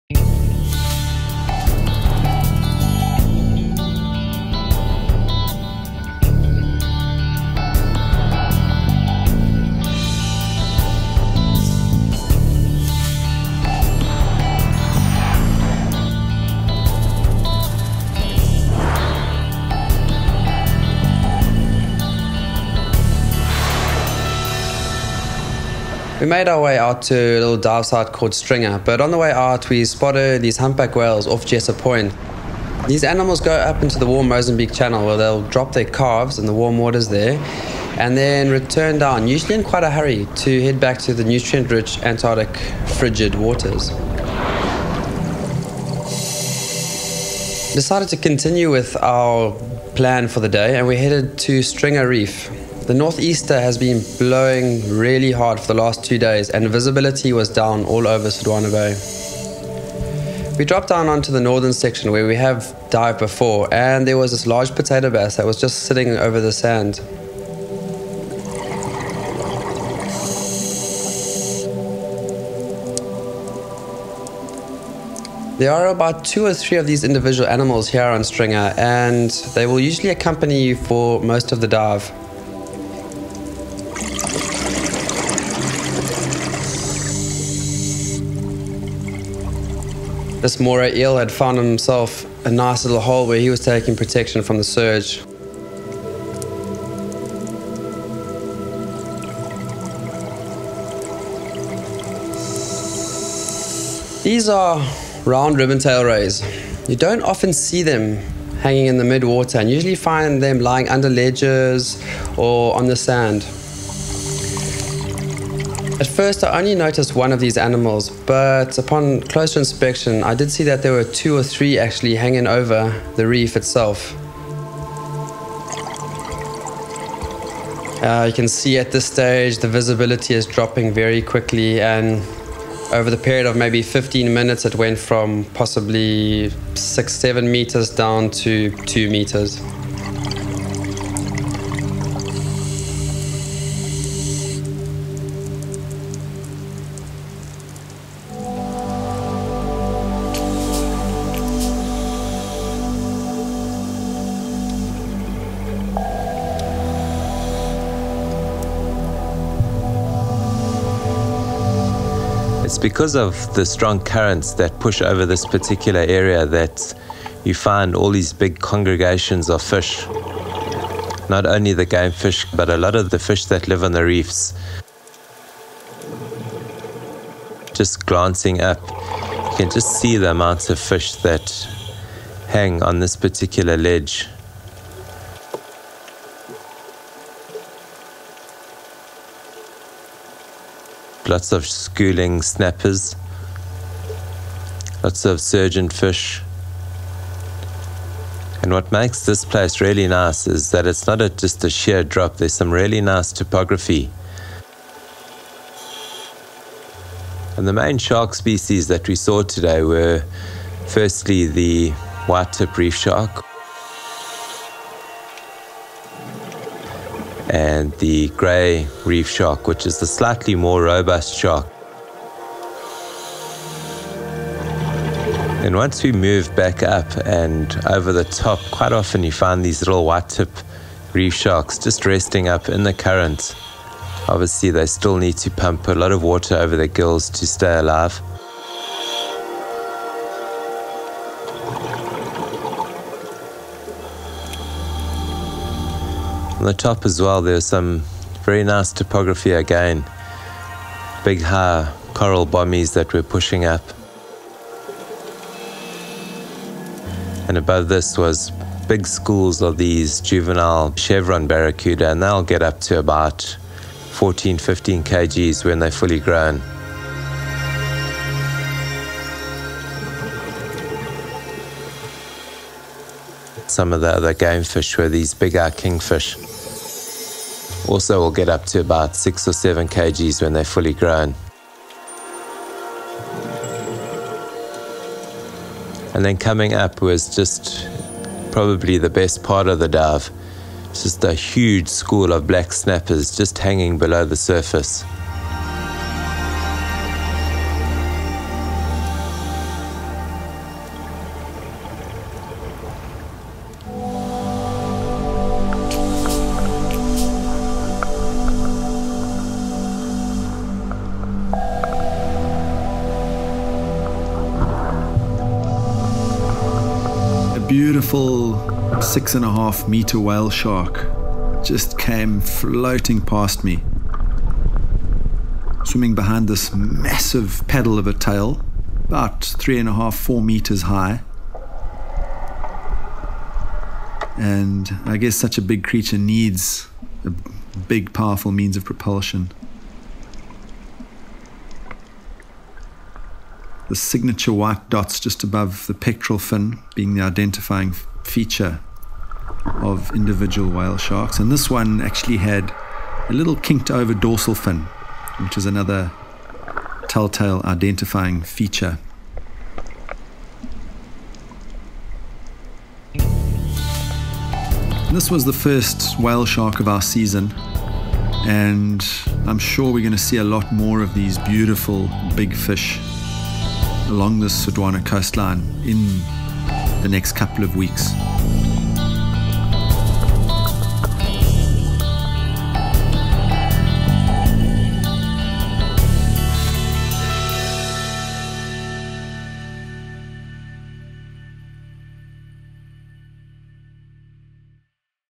Whales on the move & the biggest fish on earth
Check out the ocean's biggest animals: whales on the move & a 6-metre whale shark (05:18) - the biggest fish on earth! The Earth-Touch crew also explores amazing reef environments - see a friendly potato bass, sharks, stingrays & a lurking eel.